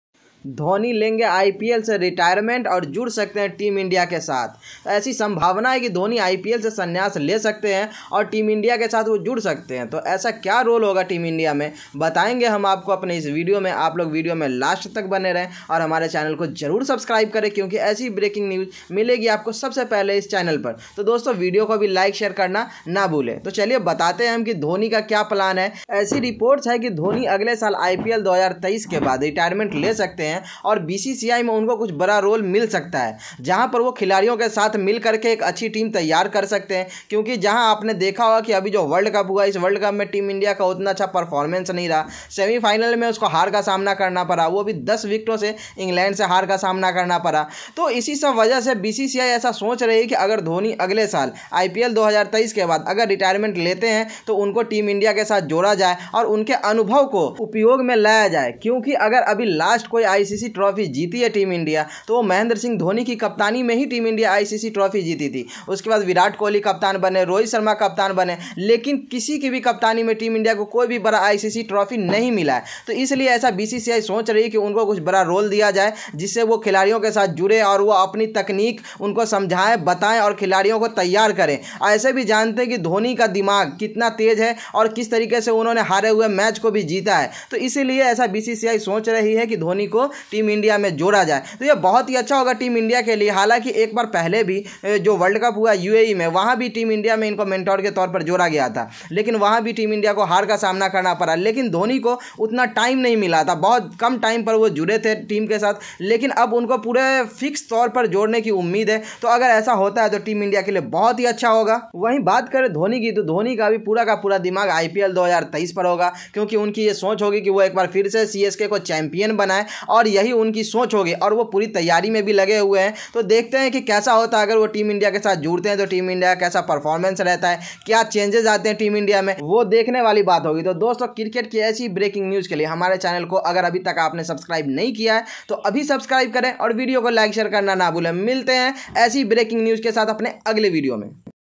IPL 2023 : धोनी लेंगे आईपीएल से संन्यास, जुड़ सकते हैं टीम इंडिया के साथ, मिलेगा बड़ा रोल टीम में
धोनी ले सकते हैं आईपीएल से रिटायरमेंट, जुड़ सकते हैं टीम इंडिया के साथ, मिल सकता है बड़ा रोल टीम इंडिया में ।